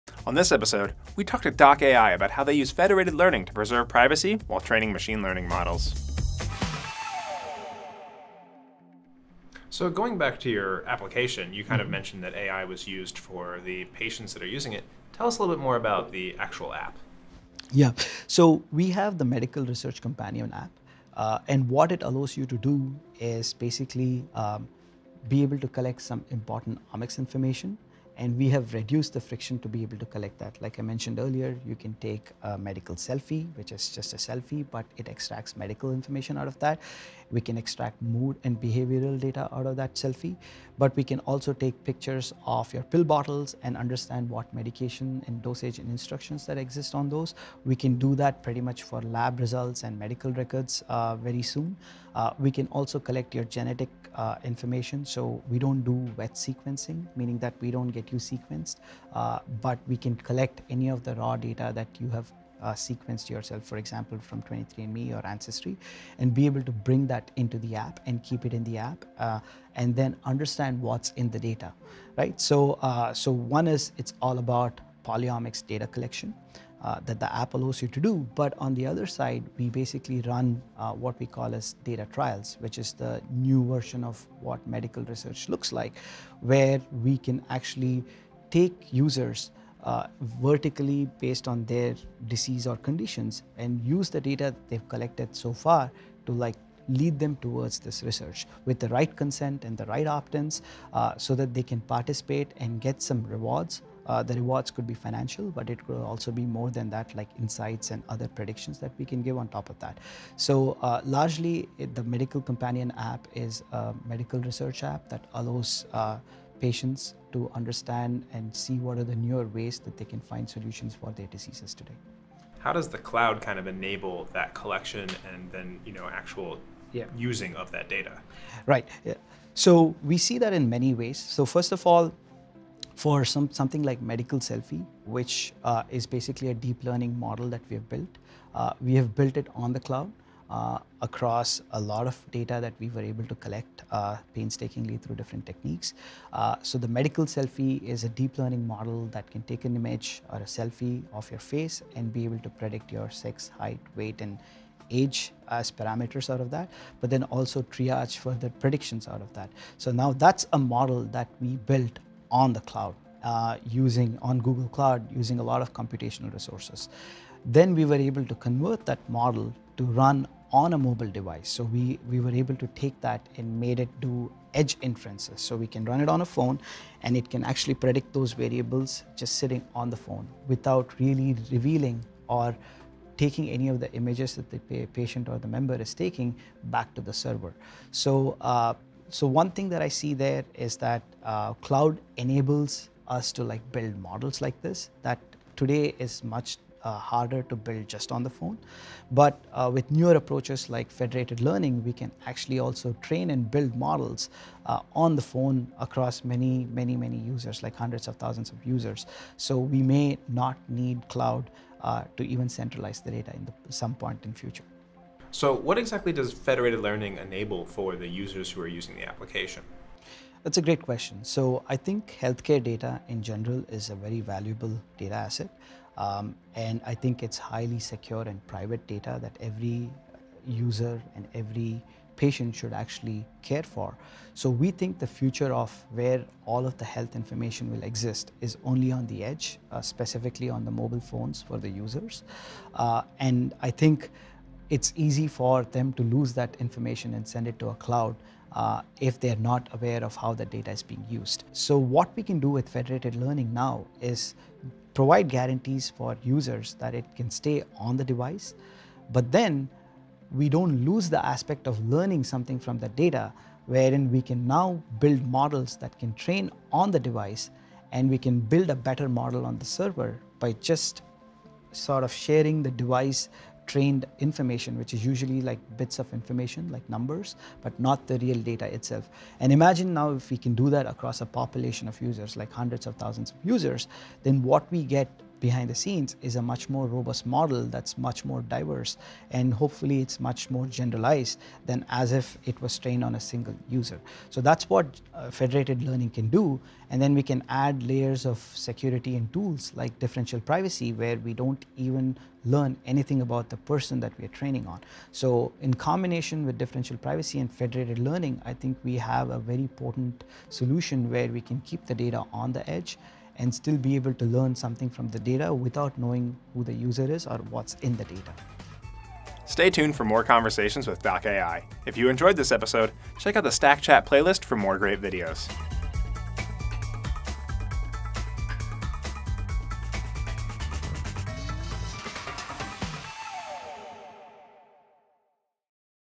Privacy and Federated Learning with doc.ai
In this episode, Mark and Akshay of doc.ai chat about federated learning and how it enables users to keep their healthcare data private, while still influencing custom models.

Product: Google Cloud Platform; fullname: Mark Mirchandani, Akshay Sharma;